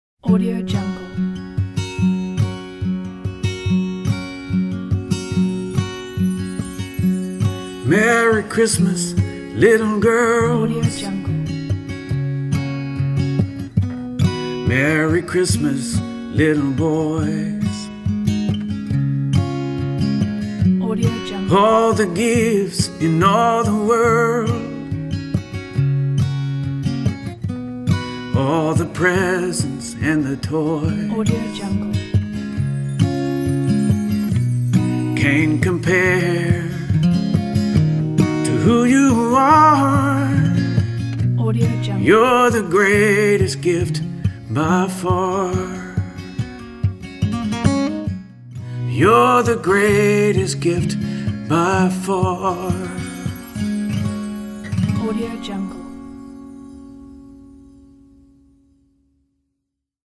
Festive Paper Cutouts Gallery
A creative paper cutout template for the festive period -- ideal for a Christmas slideshow of your family photos or videos.

This simple and elegant album template incorporates unique festive illustrations in a paper cutout style.

Features :

• No plugins required • Up to 48 images/video and text placeholders (comps ready for 12, 24, 36 and 48) • Title and end slides • HD 1080p